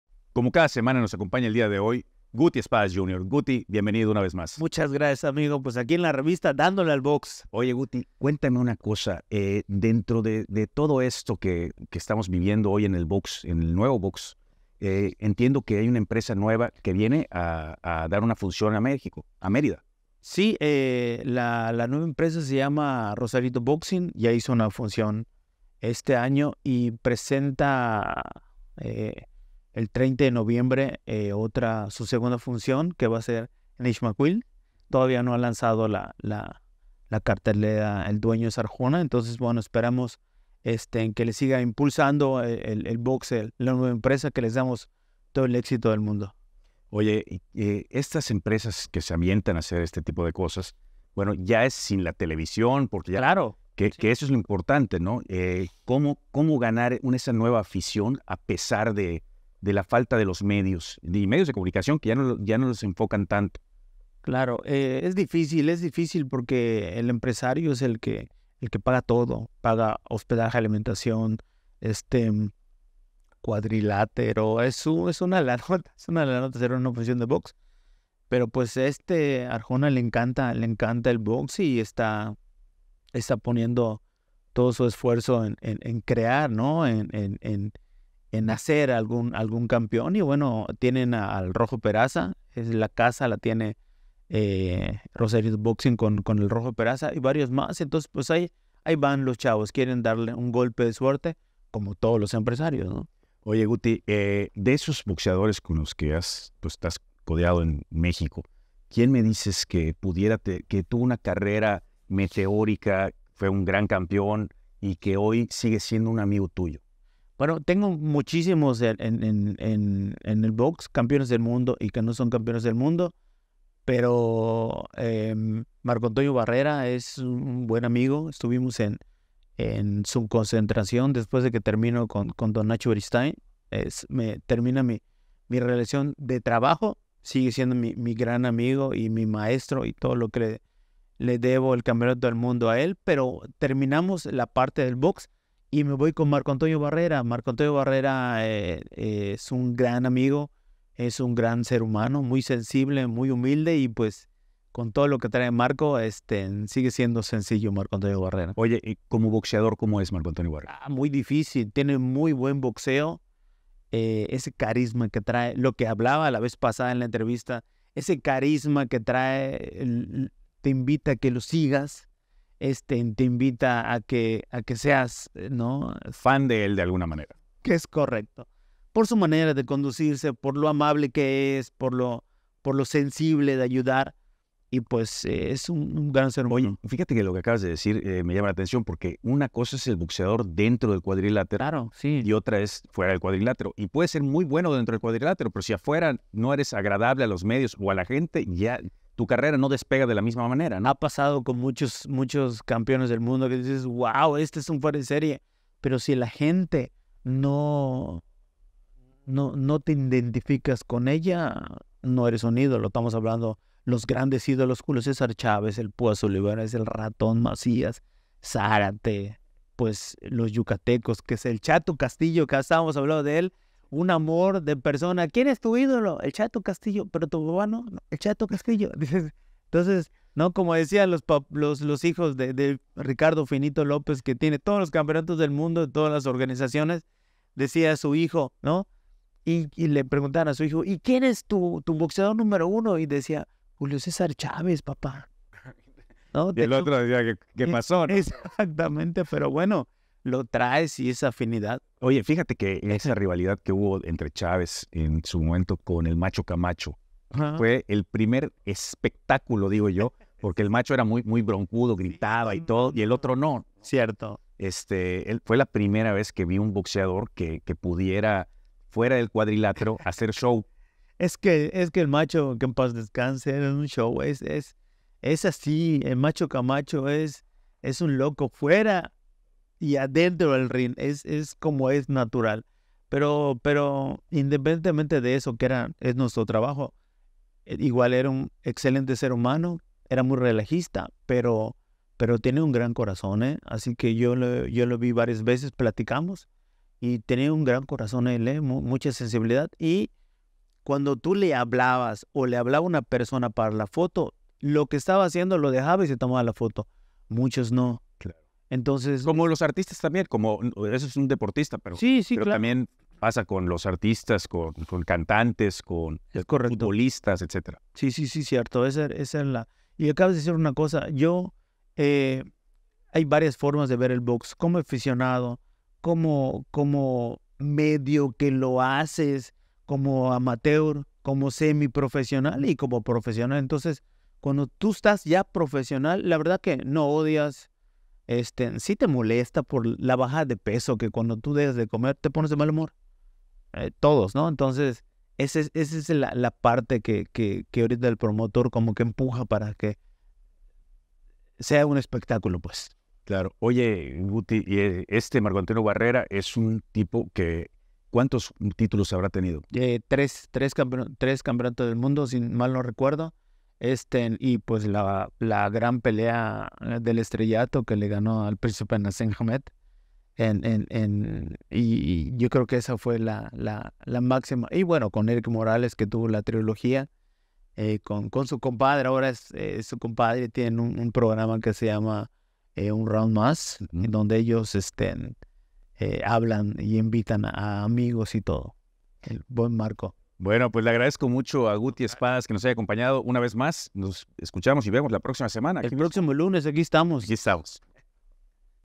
Rosarito Boxing Revoluciona el Boxeo en Yucatán - Guty Espadas Jr.
Explora el impacto de Rosarito Boxing en el boxeo yucateco con la visión de Guty Espadas Jr. 🥊

👊 En esta entrevista exclusiva, abordamos:
- Rosarito Boxing: La nueva cara del boxeo en Yucatán.
- El compromiso de Guty Espadas Jr. con el futuro del boxeo regional.
- Estrategias para ganar aficionados sin el apoyo de la TV.

📅 Próximo evento: 20 de noviembre